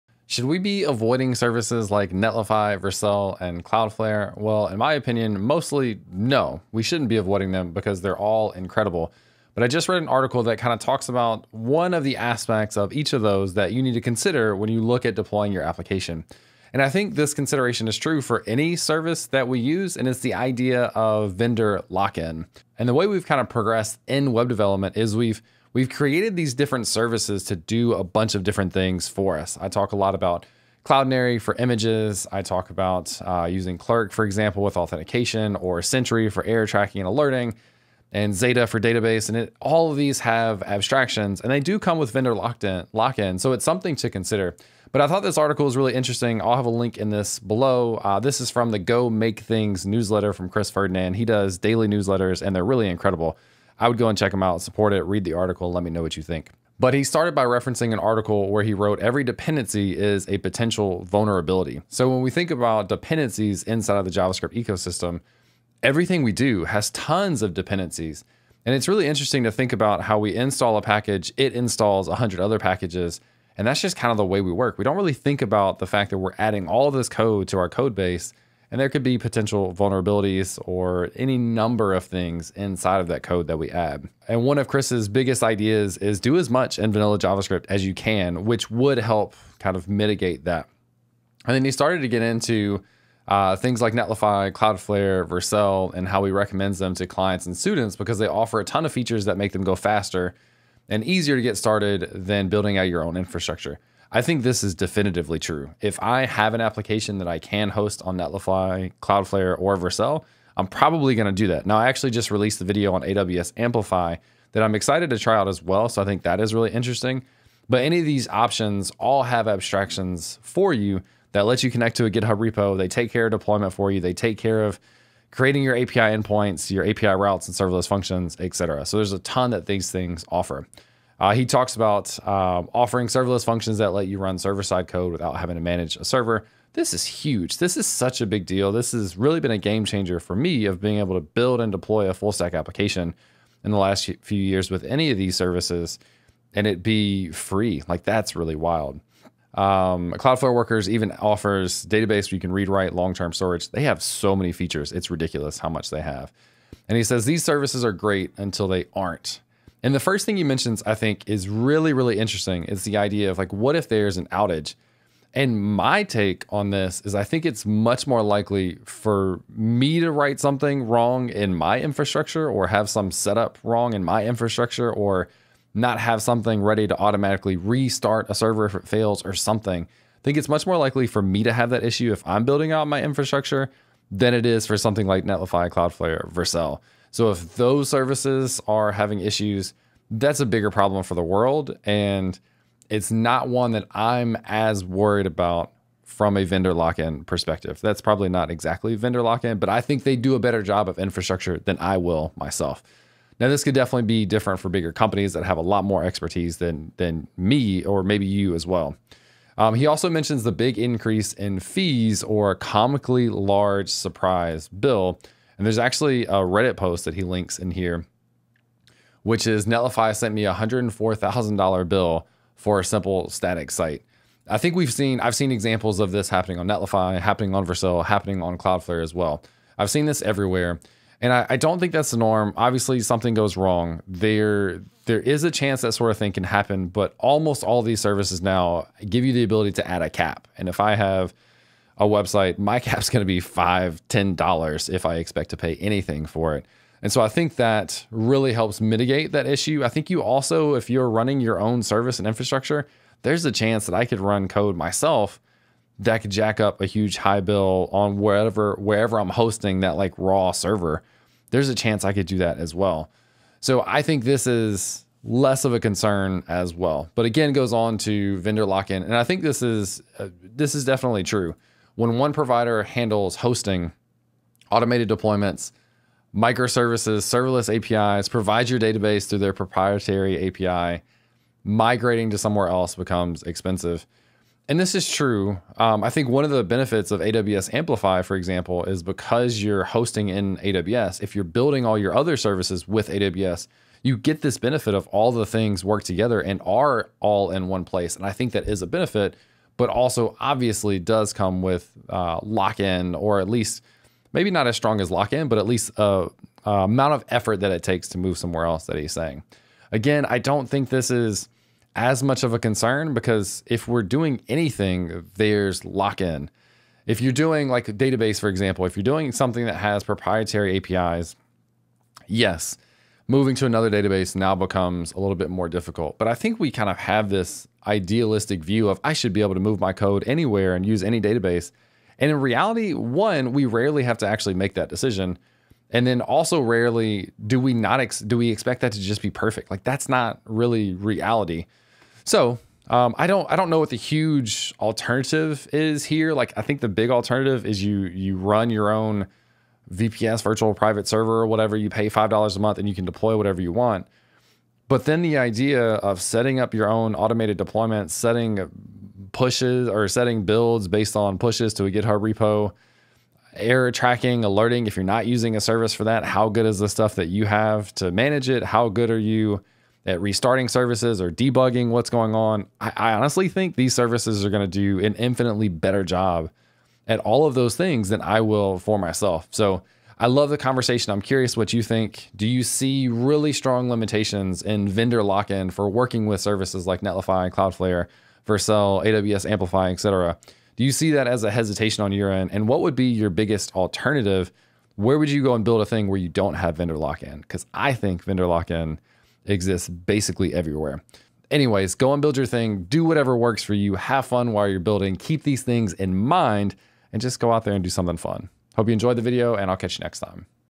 Should You Avoid Netlify, Vercel, and Cloudflare? (Quick Reaction)
Should you be managing your own hosting instead of using hosting providers like Netlify, Vercel, an Cloudflare? I think not but this article suggests you should.

**QUESTIONS ABOUT MY SETUP**
Check out my Uses page for my VS Code setup, what recording equipment I use, etc.

**TIMESTAMPS**
00:00 - Intro
01:30 - Why I Love Netlify, Vercel, and Cloudflare
02:39 - When Are Hosting Platforms Not Great
06:00 - Wrap Up